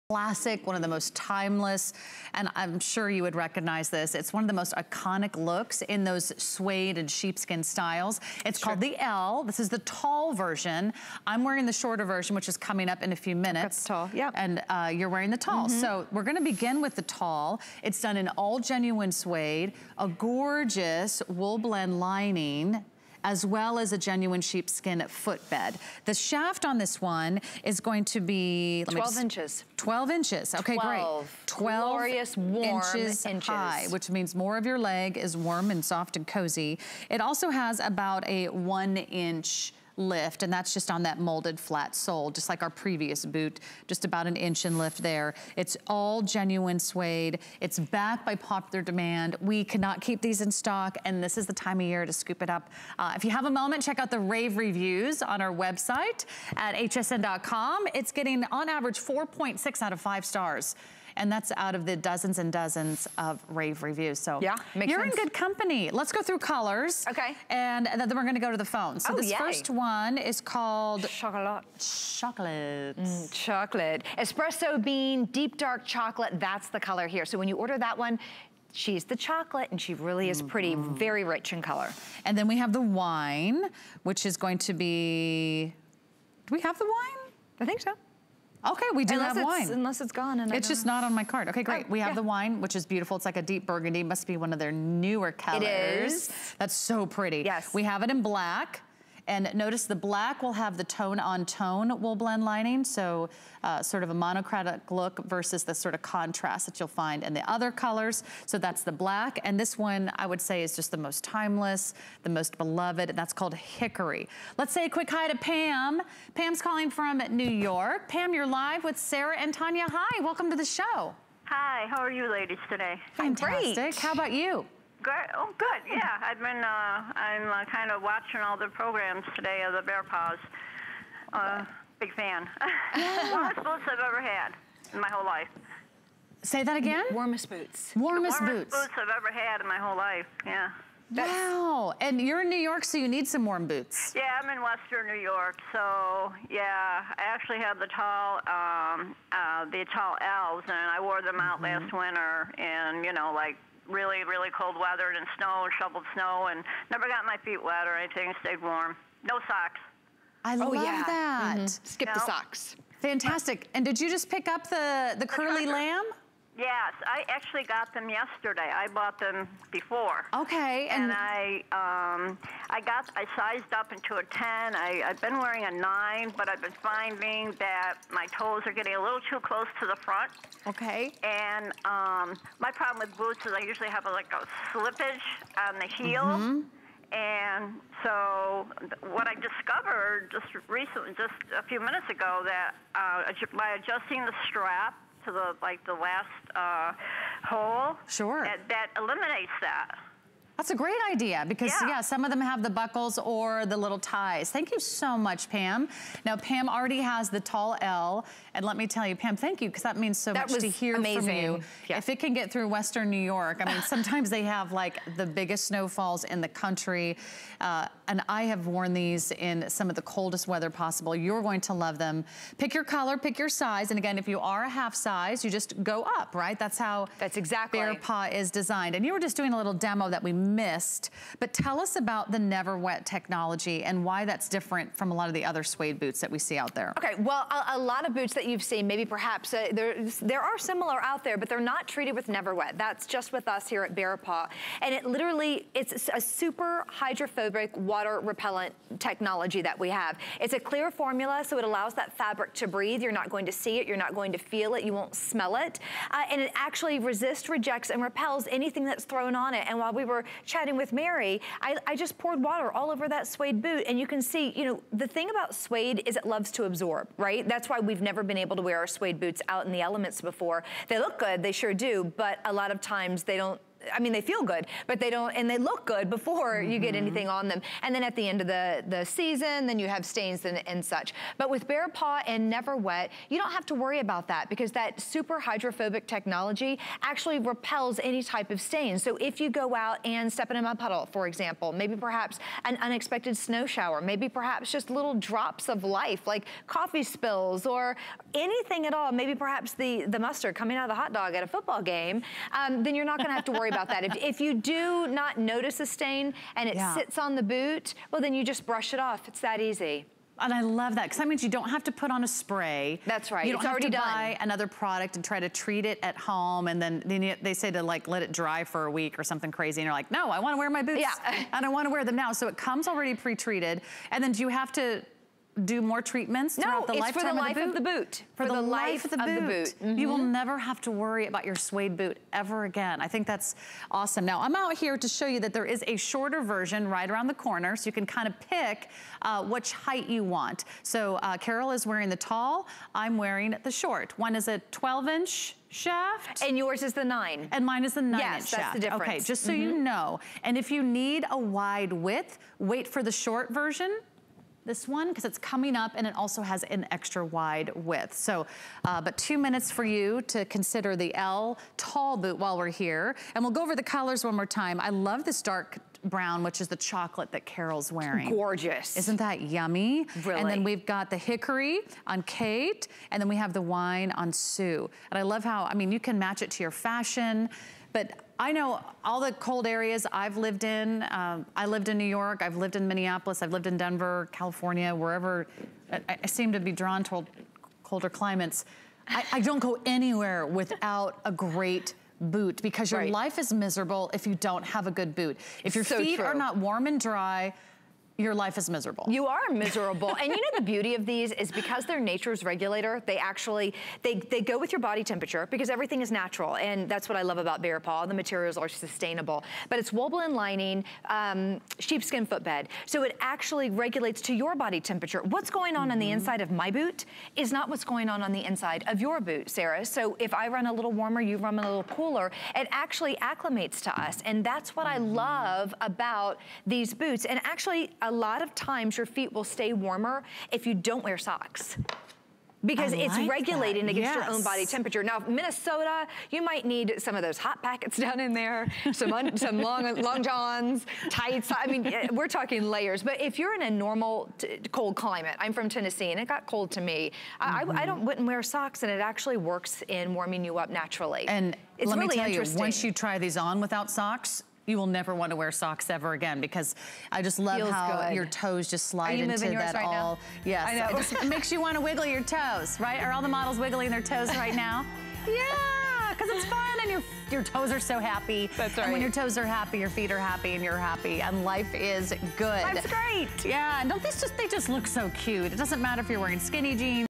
BEARPAW Elle Tall Suede Sheepskin Boot with NeverWet
This classic suede boot features a slimmeddown construction with the comfortable fit BEARPAW is known for. Treated with NeverWet technology,...